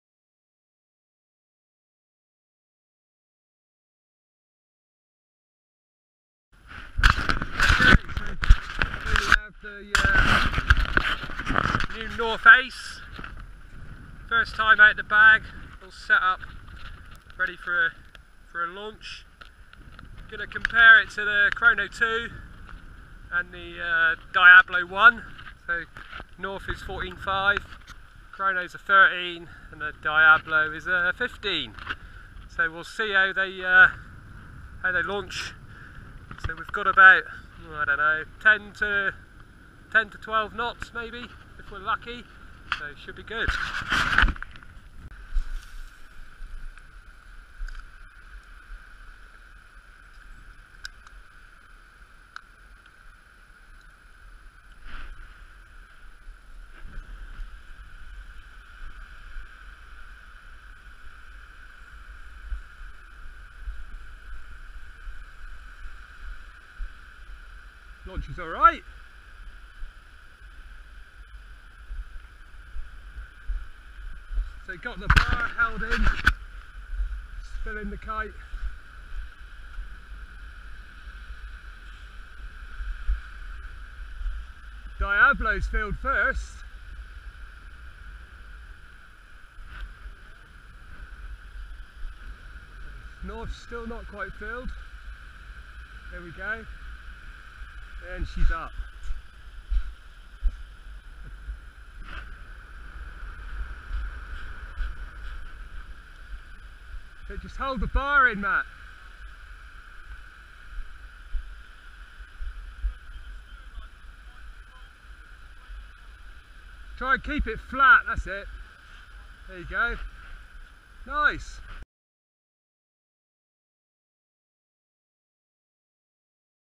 North Ace vs Chrono 2 & Diablo - Part 1 The Launch
North Ace 14.5 Foil kite lining up against Ozone Chrono 2 & FOne Diablo.
Location- Marazion, Cornwall
Rider - Lee Pasty Harvey
Wind - 8-10 knts

Part 2 - On the water. Coming soon!